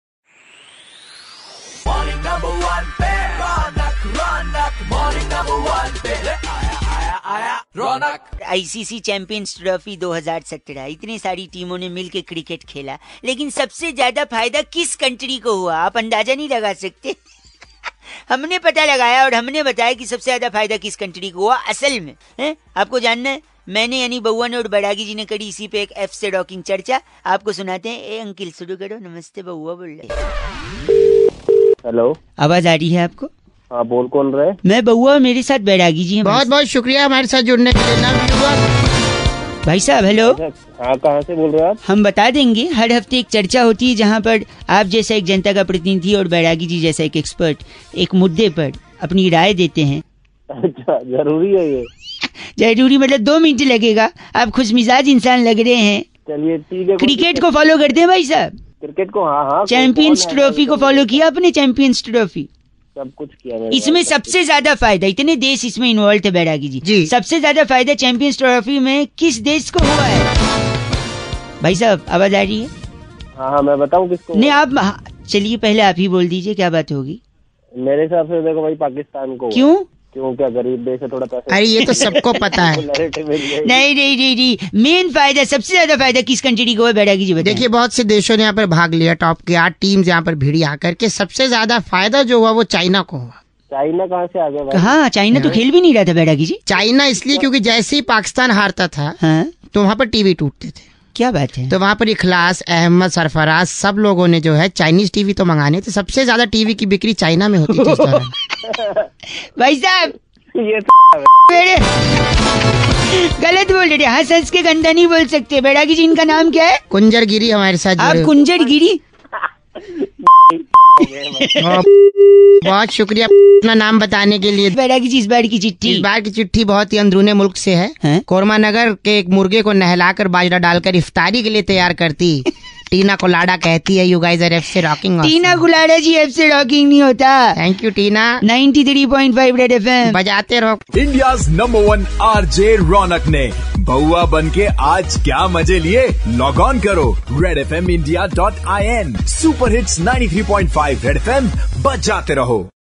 BAUAA AND BAIRAGI CHARCHA ON CRICKET CHINA KO BHAUT NUKSAAN HUA
Baua aur Nand Kishore Bairagi Charcha mein.

Is baar ka topic hai Cricket se china ko bahot nuksan hua hai.